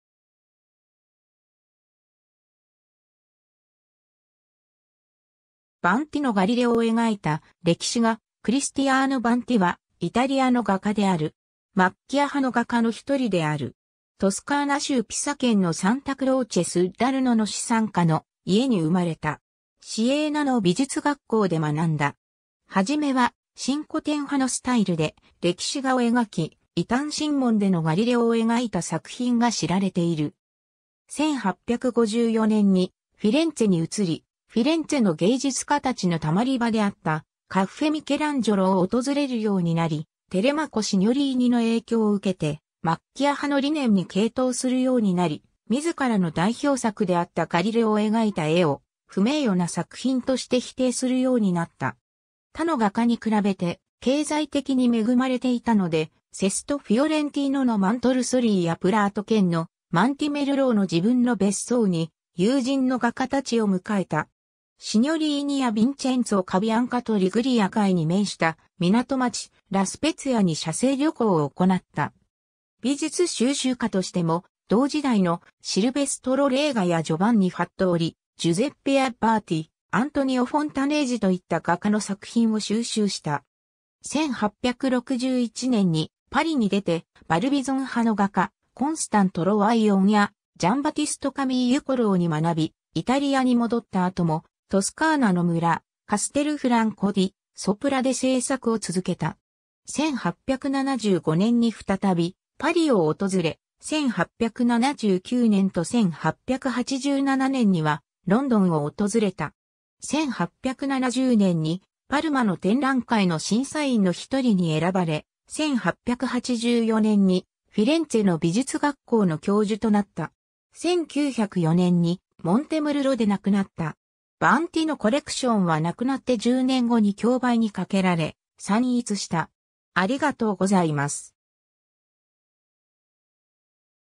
クリスティアーノ・バンティ
バンティのガリレオを描いた歴史画 (1857) クリスティアーノ・バンティ（Cristiano Banti 、1824年1月4日 - 1904年12月4日）はイタリアの画家である。
「マッキア派」の画家の一人である。
 トスカーナ州ピサ県のサンタ・クローチェ・スッラルノの資産家の家に生まれた。
シエーナの美術学校で学んだ。
初めは「新古典派」のスタイルで歴史画を描き、異端審問でのガリレオを描いた作品が知られている。
 1854年にフィレンツェに移り、フィレンツェの芸術家たちのたまり場であった「カッフェ・ミケランジョロ」を訪れるようになり、テレマコ・シニョリーニの影響を受けて、「マッキア派」の理念に傾倒するようになり、自らの代表作であったガリレオを描いた絵を「不名誉な作品」として否定するようになった。
他の画家に比べて経済的に恵まれていたので、セスト・フィオレンティーノのMontorsoliやプラート県のMontemurloの自分の別荘に友人の画家たちを迎えた 。
シニョリーニやヴィンチェンツォ・カビアンカとリグリア海に面した港町、ラ・スペツィアに写生旅行を行った。
美術収集家としても同時代の、シルヴェストロ・レーガやジョヴァンニ・ファットーリ、ジュゼッペ・アッバーティ、アントニオ・フォンタネージといった画家の作品を収集した。
 1861年にパリに出て、「バルビゾン派」の画家、コンスタン・トロワイヨンやジャン＝バティスト・カミーユ・コローに学び、イタリアに戻った後もトスカーナの村、カステルフランコ・ディ・ソプラで製作を続けた。
1875年に再びパリを訪れ、1879年と1887年にはロンドンを訪れた。
 1870年にパルマの展覧会の審査員の一人に選ばれ、1884年にフィレンツェの美術学校(Accademia di belle arti di Firenze)の教授となった 。
 1904年にモンテムルロで亡くなった。
バンティのコレクションは亡くなって10年後に競売にかけられ散逸した。